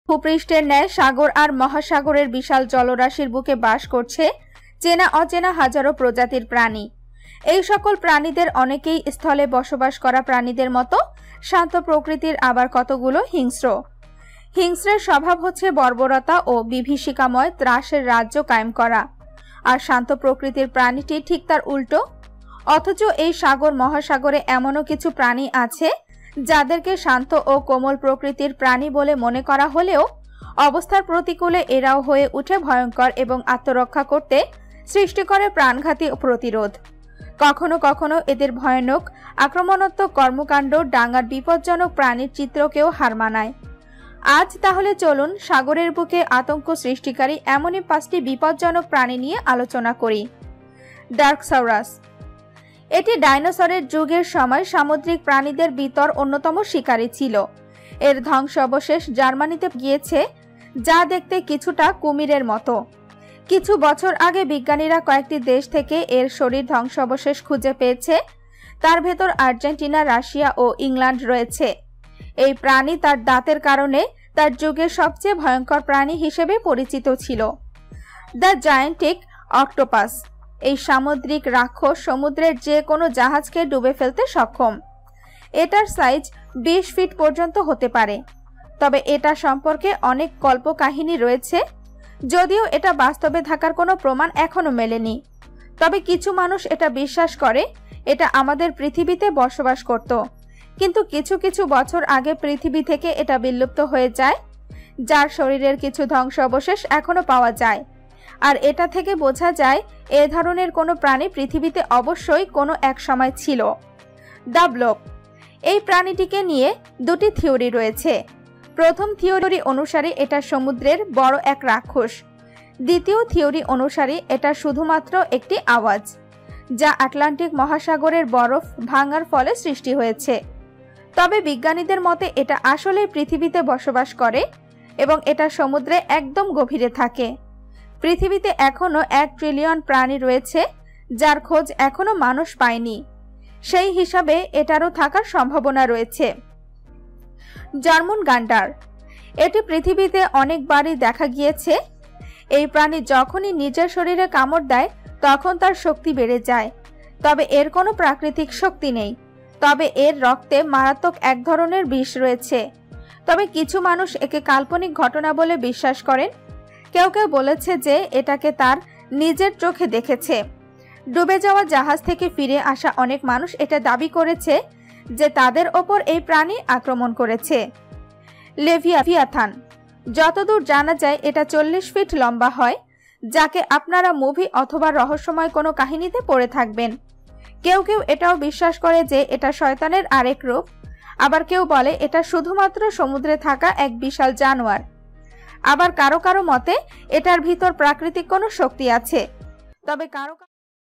সাগরের বুকে আতঙ্ক সৃষ্টিকারী এমনি ৫টি বিপজ্জনক প্রাণী যা দেখলে অবাক না হয়ে পারবেন না।
এই পৃথিবীর ভূপৃষ্ঠের ন্যায় সাগর আর মহাসাগরের বিশাল জলরাশির বুকে বাস করছে চেনা অচেনা হাজারো প্রজাতির প্রাণী। এ সকল প্রানীদের অনেকেই স্থলে বসবাস করা প্রানীদের মত শান্ত প্রকৃতির আবার কতকগুলো হিংস্র। হিংস্রের স্বভাব হচ্ছে বর্বরতা ও বিভীষিকাময় ত্রাসের রাজ্য কায়েম করা। আর শান্ত প্রকৃতির প্রাণীটি ঠিক তার উল্টো। অথচ এই সাগর - মহাসাগরে এমনও কিছু প্রাণী আছে যাদেরকে শান্ত ও কোমল প্রকৃতির প্রাণী বলে মনে হলেও অবস্থার প্রতিকূলে এরাও হয়ে উঠে ভয়ংকর এবং আত্মরক্ষা করতে সৃষ্টিকরে প্রাণঘাতী প্রতিরোধ। কখনো কখনো এদের ভয়ানক আক্রমণাত্মক কর্মকাণ্ড ডাঙ্গার বিপজ্জনক প্রাণীর চরিত্রকেও হার মানায়। আজ তাহলে চলুন সাগরের বুকে আতঙ্ক সৃষ্টিকারী এমনি ৫টি বিপজ্জনক প্রাণী নিয়ে আলোচনা করি।

Darkosaurus

এটি ডাইনোসরের যুগ এর সময় সামুদ্রিক প্রাণীদের বিতর অন্যতম শিকারি ছিল। এর ধ্বংসাবশেষ জার্মানিতে পাওয়া গিয়েছে। যা দেখতে কিছুটা কুমিরের মতো। কিছু বছর আগে বিজ্ঞানীরা কয়েকটি দেশ থেকে এর শরীর ধ্বংসাবশেষ খুঁজে পেয়েছে। তার ভিতর আর্জেন্টিনা রাশিয়া ও ইংল্যান্ড রয়েছে। এই প্রাণী তার দাঁতের কারণে তার যুগে সবচেয়ে ভয়ঙ্কর প্রাণী হিসেবে পরিচিত ছিল।

The Gigantic Octopus
এই সমুদ্রিক রাক্ষস সমুদ্রের যে কোন জাহাজকে ডুবে ফেলতে সক্ষম। এটার সাইজ 20 ফিট পর্যন্ত হতে পারে।। তবে এটা সম্পর্কে অনেক কল্পকাহিনী রয়েছে। যদিও এটা বাস্তবে থাকার কোন প্রমাণ এখনও মেলেনি। তবে কিছু মানুষ এটা বিশ্বাস করে, এটা আমাদের পৃথিবীতে বসবাস করত। কিন্তু কিছু কিছু বছর আগে পৃথিবী থেকে এটা বিলুপ্ত  হয়ে যায়। যার শরীরের কিছু ধ্বংসাবশেষ এখনো পাওয়া যায়। আর এটা থেকে বোঝা যায় এ ধরনের কোনো প্রানী পৃথিবীতে অবশ্যই কোনো এক সময় ছিল।